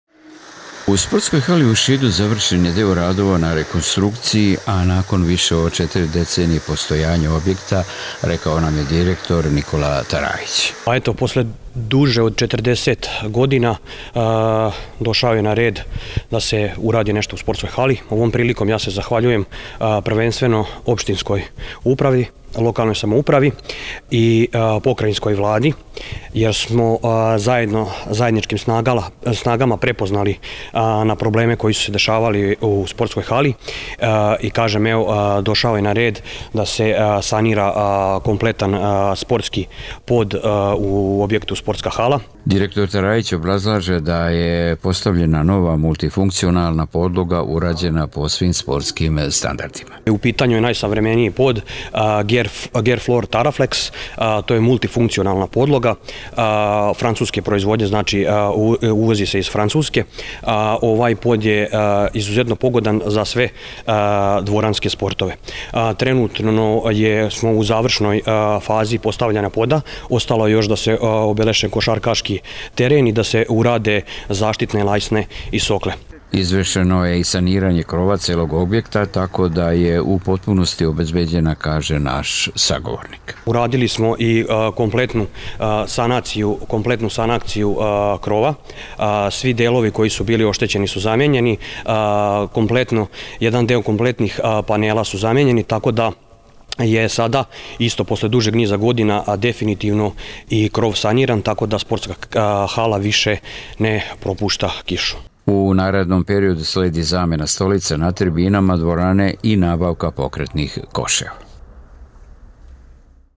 Rekonstrukcija hale u Sidu
Pokrajinska Vlada i opština Šid obezbedili su potrebna finansijska sredstva za rekonstrukciju sportske hale u Šidu, a povaj posao izvršen je nakon više od četri decenije postojanja objekta. Postavljene su nove podloge u dvorani koja je sada urađena po potrebnim sportskim standarima.